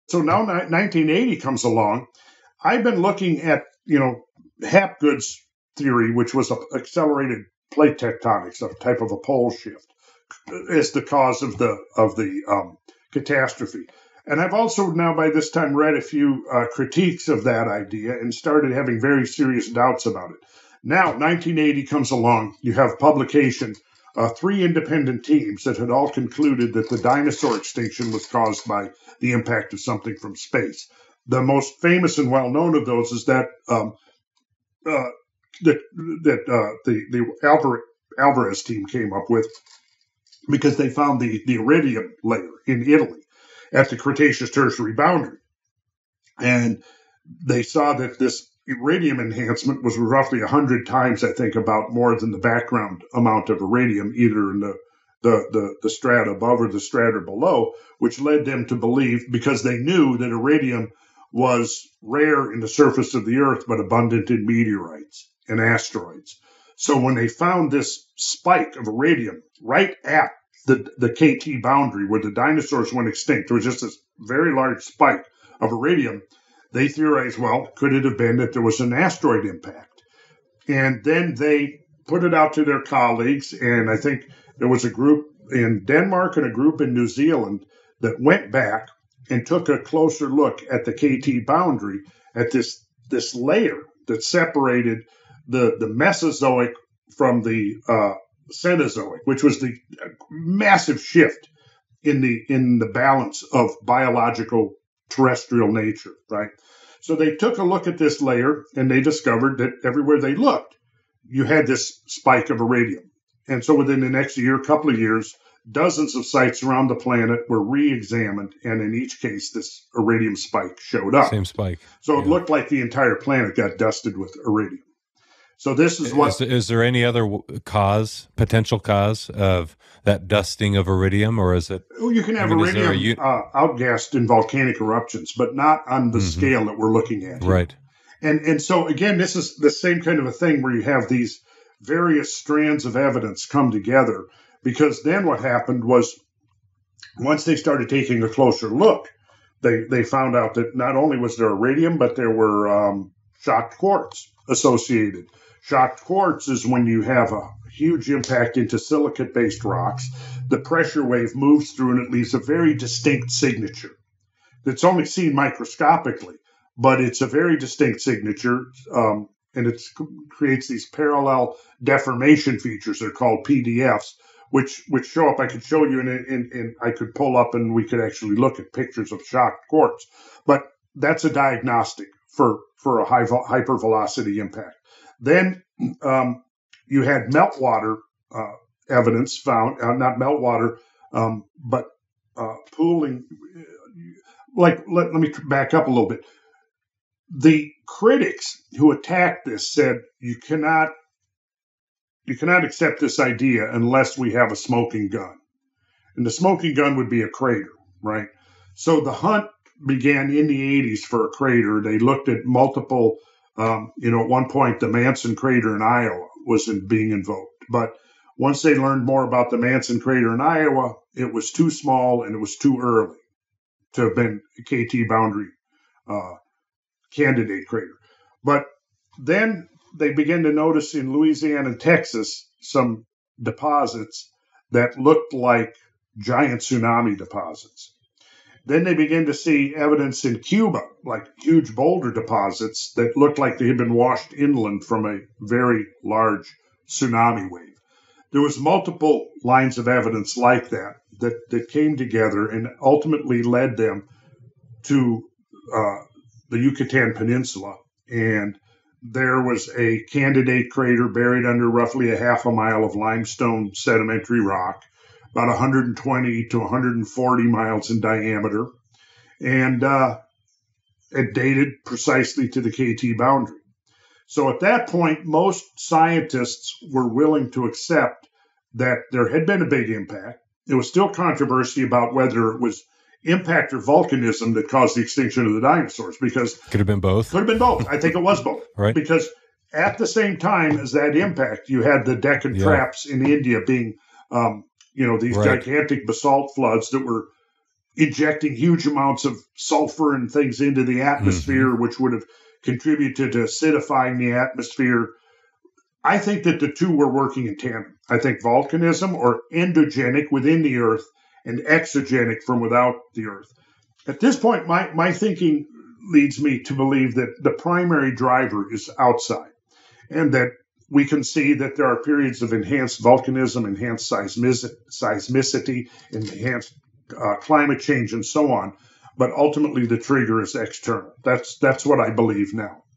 Randall Carlson on the True Cause of Dinosaur Extinction
By 1980, the scientific consensus about the dinosaurs' extinction was that it was caused by an impact of something that came from space.

However, there are still critics of this theory today and much controversy about how the dinosaurs became extinct. Does the asteroid theory actually hold up?

Here's what Randall Carlson believes about what really happened.

Additional Resources ✅

If you’ve enjoyed this episode, please share and follow it on your favorite platform.